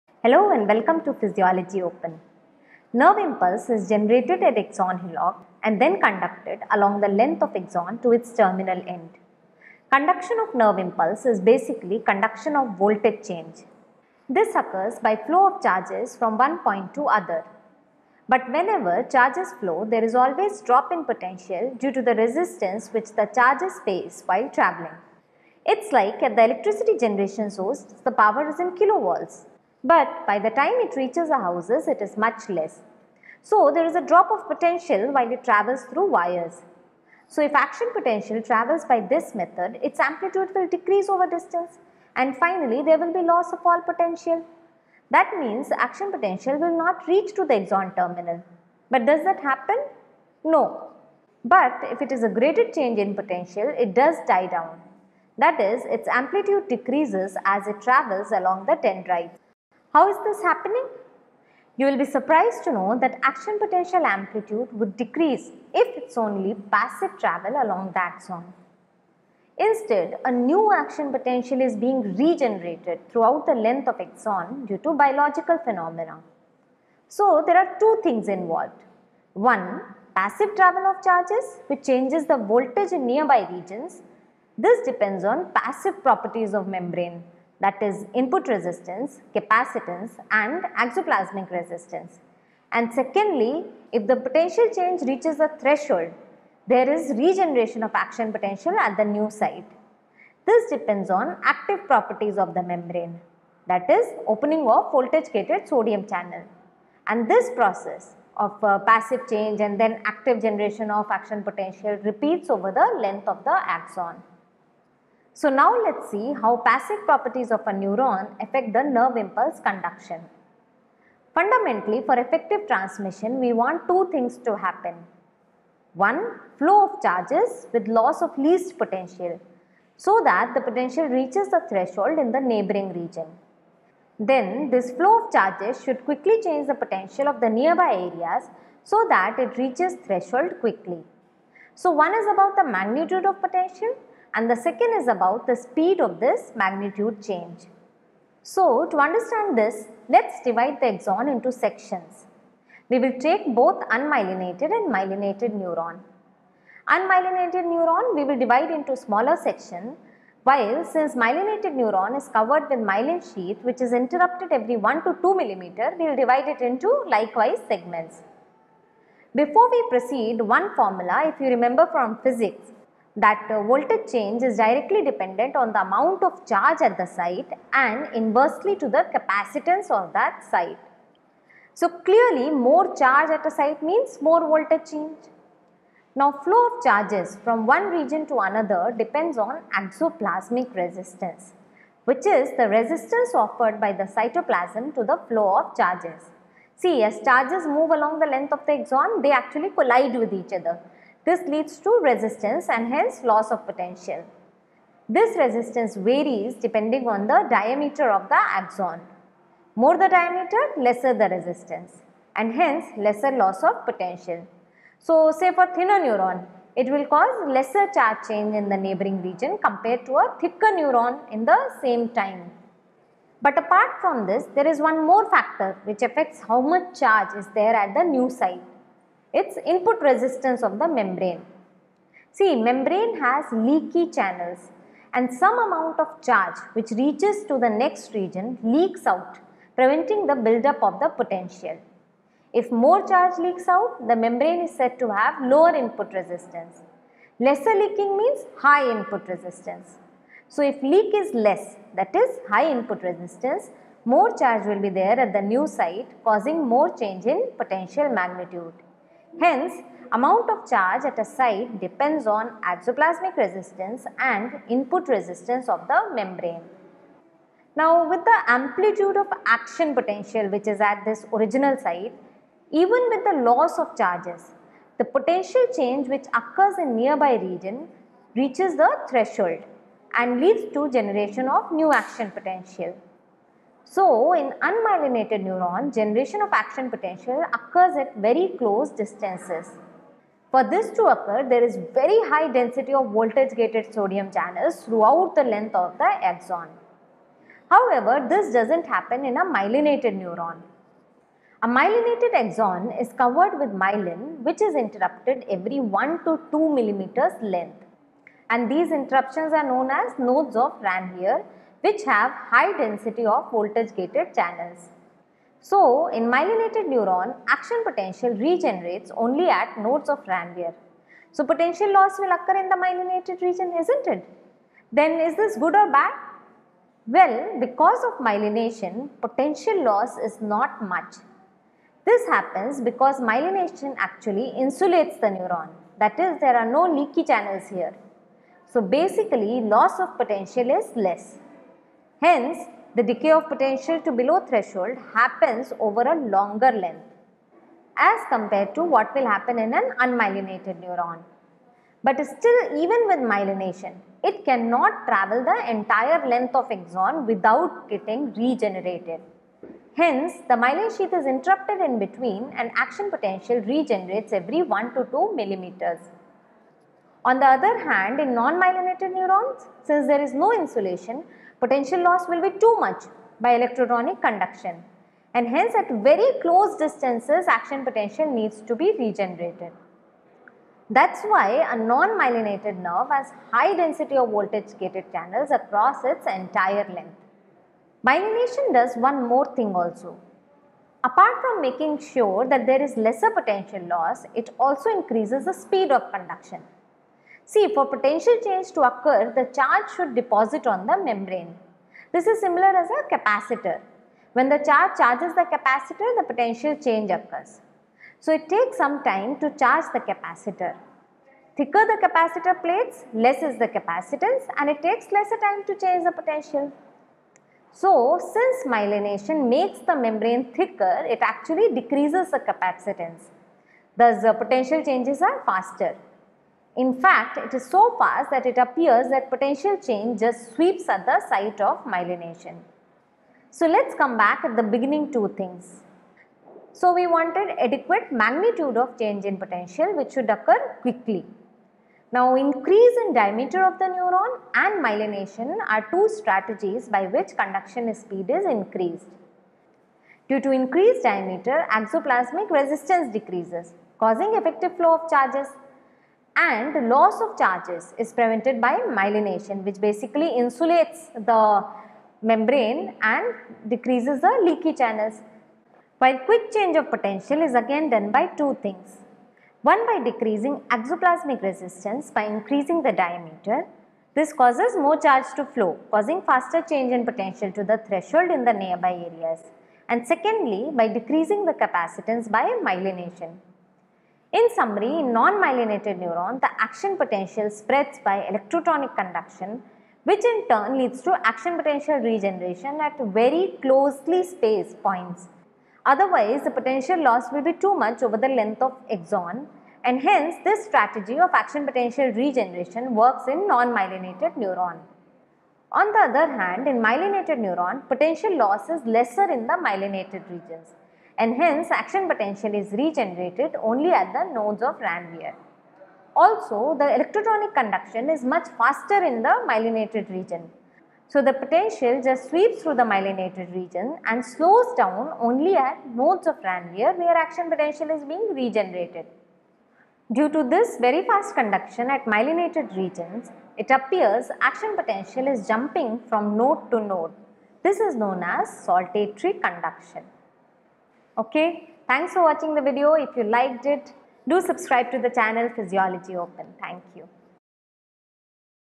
Passive electrical properties of membrane | electrotonic and saltatory conduction
Physiology lecture on nerve muscle physiology explains passive electrical properties of nerve affect conduction of action potential in a neuron; electronic conduction; saltatory conduction. How diameter and myelination affect propagation of action potential
Passive properties of nerve: Input resistance, capacitance and axoplasmic resistance